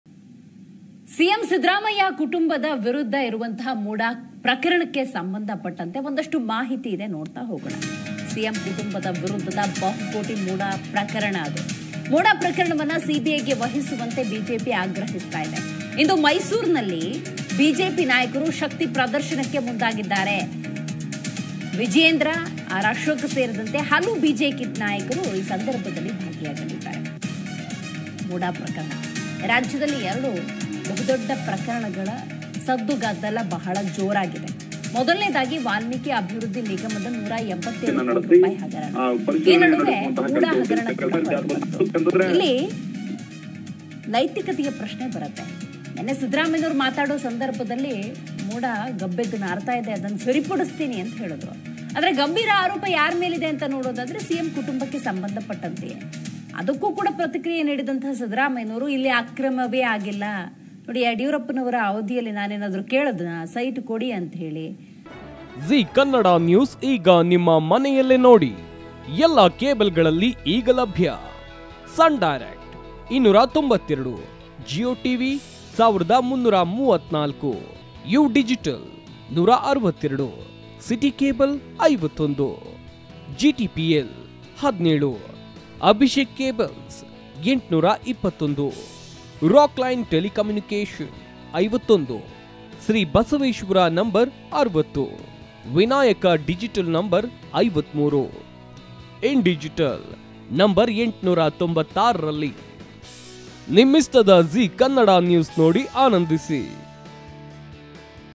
Muda Site Scam : BJP Protests In Mysuru | ಸಿಎಂ ಕುಟುಂಬದ ವಿರುದ್ಧದ ಬಹುಕೋಟಿ ಮುಡಾ ಕೇಸ್‌
ಇಂದು ಮೈಸೂರಿನಲ್ಲಿ ಬಿಜೆಪಿ ನಾಯಕರ ಶಕ್ತಿ ಪ್ರದರ್ಶನ
ವಿಜಯೇಂದ್ರ, ಅಶೋಕ್‌ ಸೇರಿದಂತೆ ಹಲವು ನಾಯಕರು ಭಾಗಿ